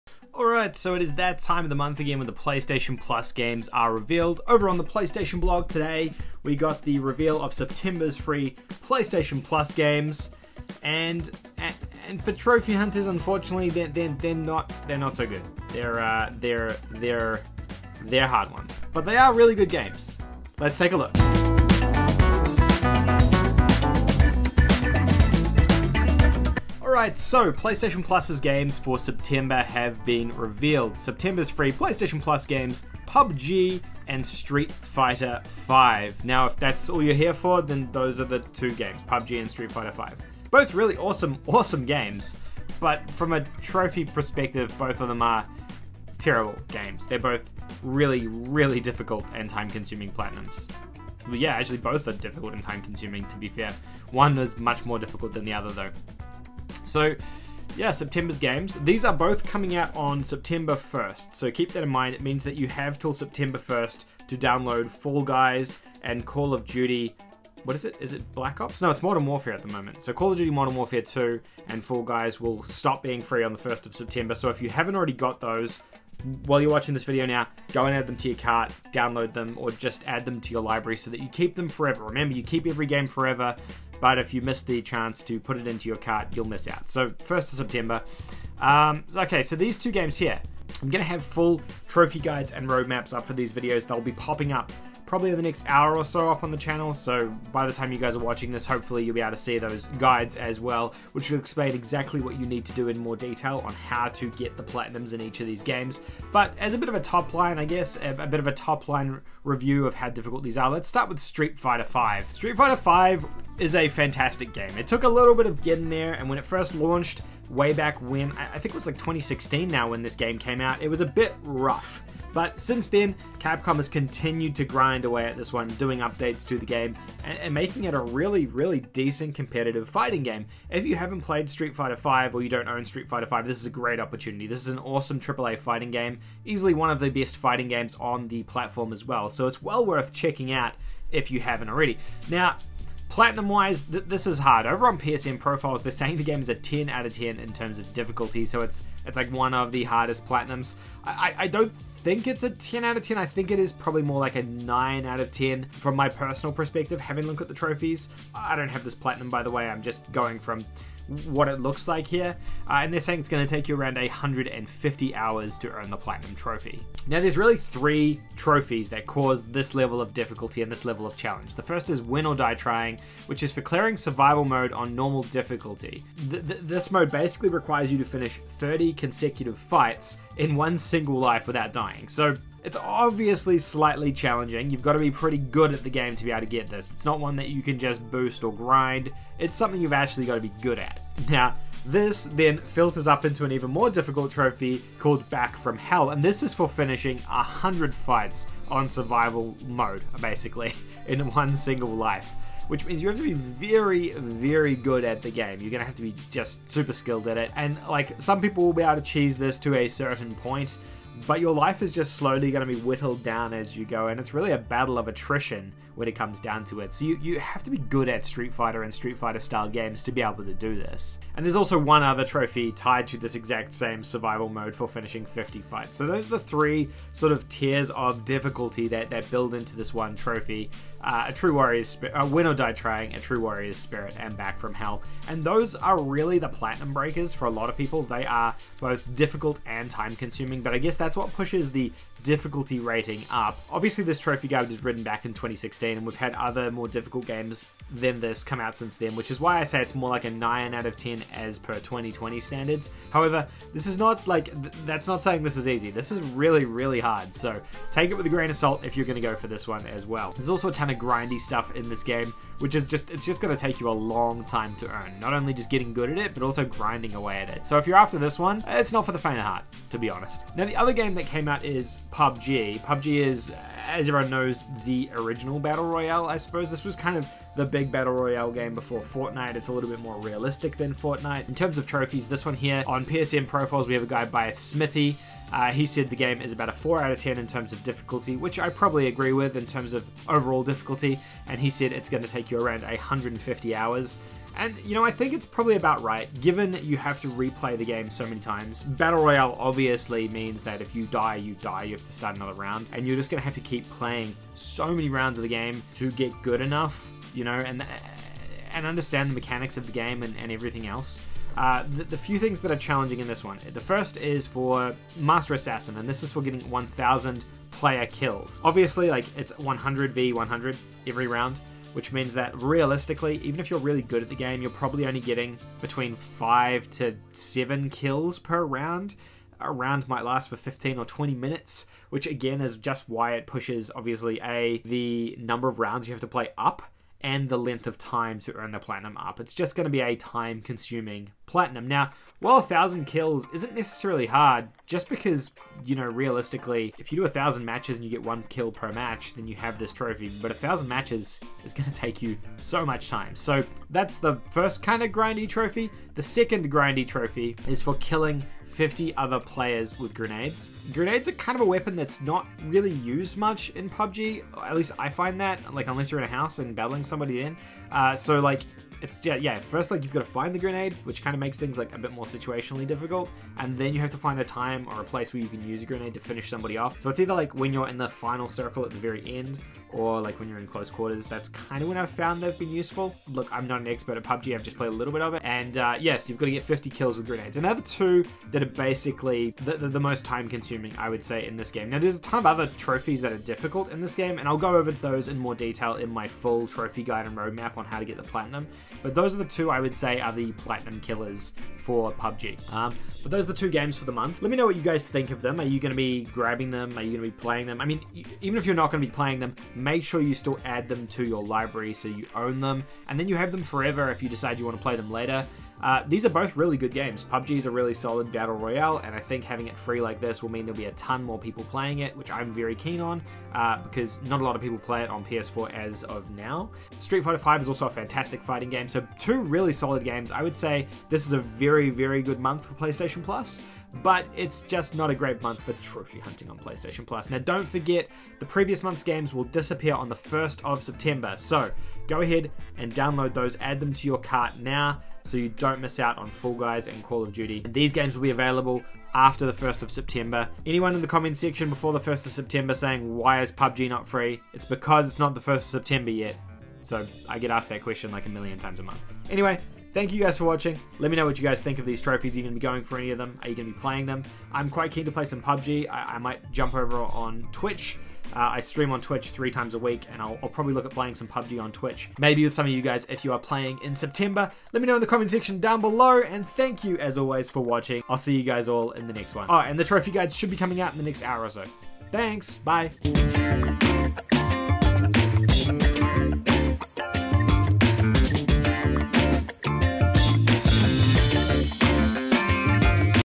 Will PubG kill Fall Guys? (September 2020 PlayStation Plus Games)
We take a look at the PlayStation Plus games for September 2020 and both of the titles are killer apps for PS plus!